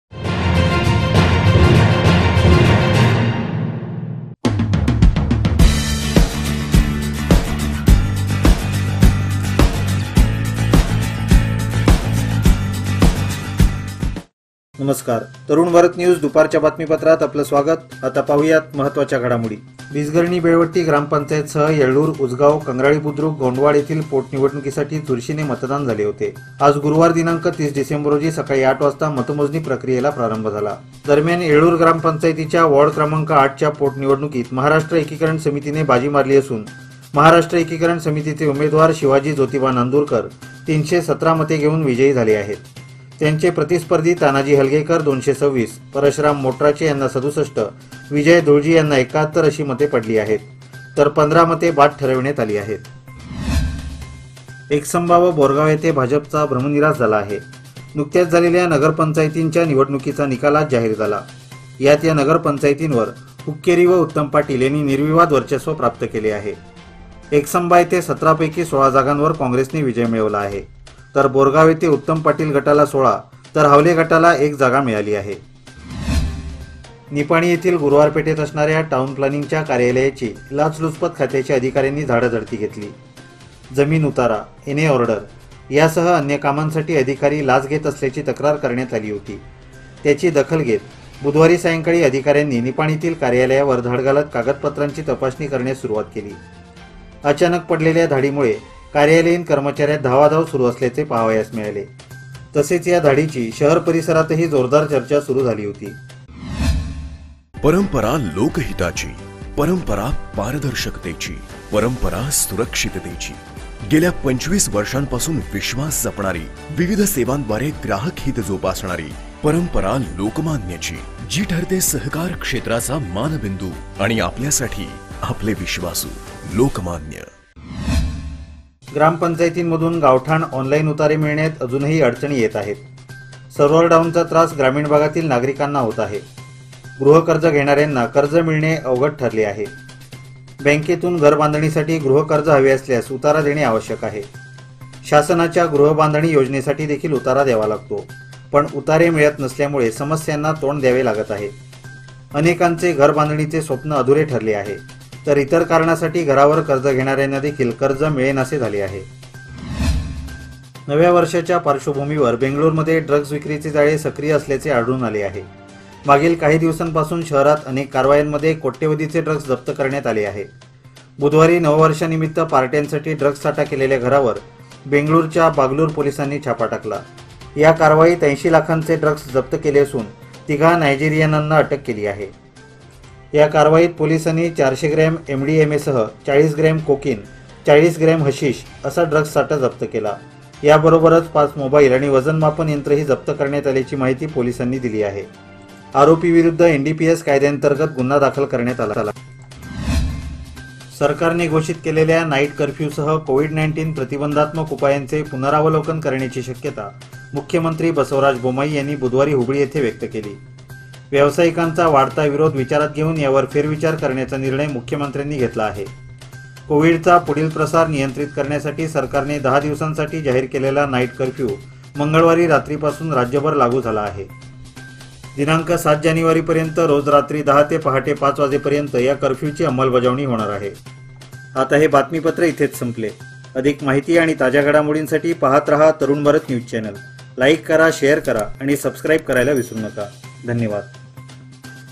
न्यूज बुलेटिन | AFTERNOON NEWS BULLETIN | 30-12-2021
१ . येळ्ळूर ग्रा. पं. पोटनिवडणूकीत म. ए. समितीची बाजी
२ . एकसंबा, बोरगावात भाजपचा भ्रमनिरास
३ . टाऊन प्लानिंग कार्यालयाची निपाणीत झाडाझडती
४ . गावठाण उतारे मिळण्यात अडचणी, सर्व्हर डाउनची समस्या कायम
५ . बेंगळूरमध्ये ड्रग्ज साठा जप्त
६ . नाईट कर्फ्यूसह  निर्बंधांवर फेरविचार ; मुख्यमंत्री बोम्माईचें संकेत

Ads : HOTEL THALI'S KITCHEN